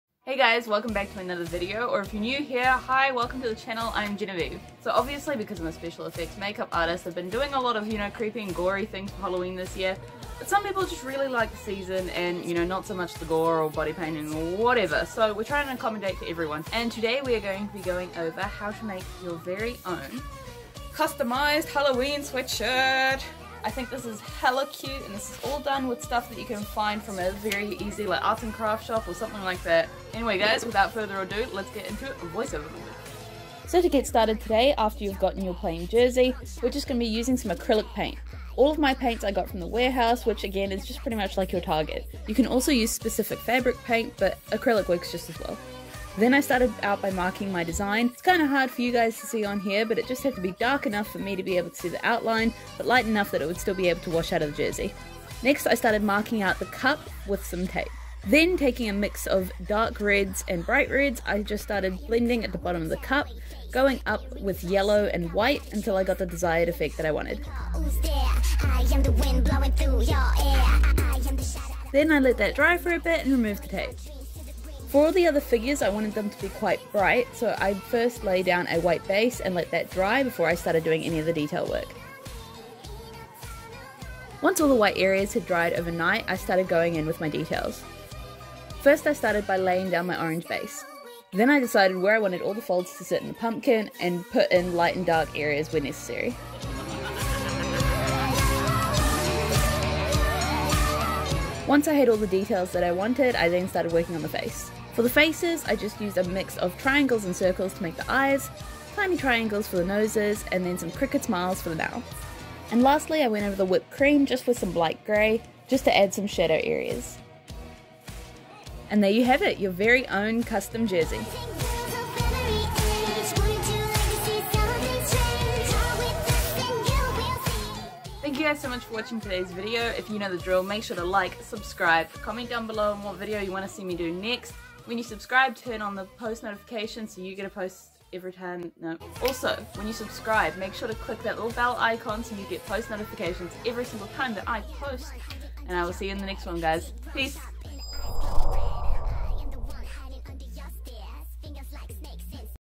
DIY Fall Inspired Halloween Sweatshirt || Acrylic Painting
Love the fall season? Create your own super comfy seasonal sweatshirt!